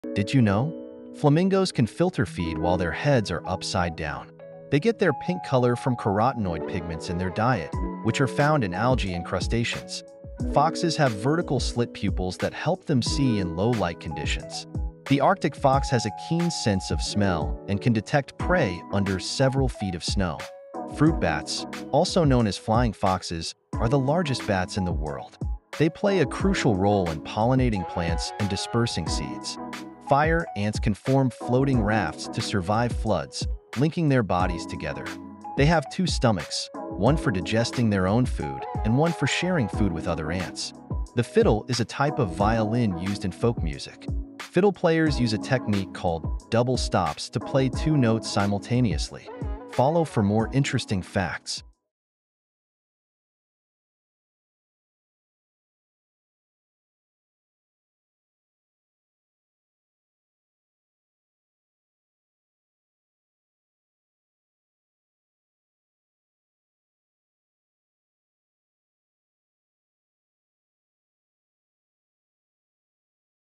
Fantastic Ferns, Fossils, and Flamingos 🌿🦴🦩
Explore fascinating facts about ferns, fossils, and flamingos! Discover ancient plants, the oldest known fossils, and the unique traits of flamingos. 🌿🦴🦩✨